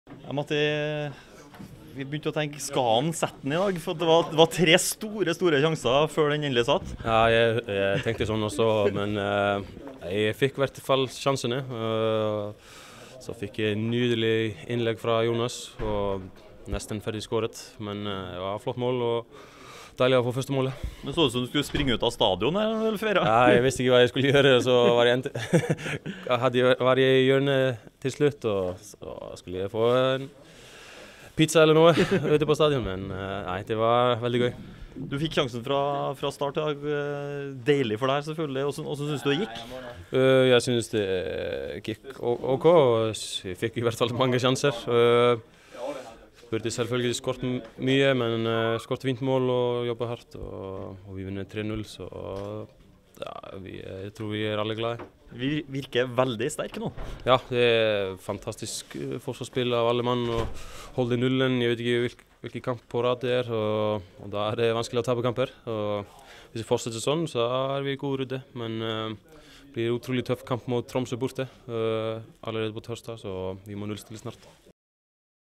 - Skulle ta meg en pizza
Matthias Vilhjalmsson sprang nesten ut av stadion etter å ha scoret kampens første mål.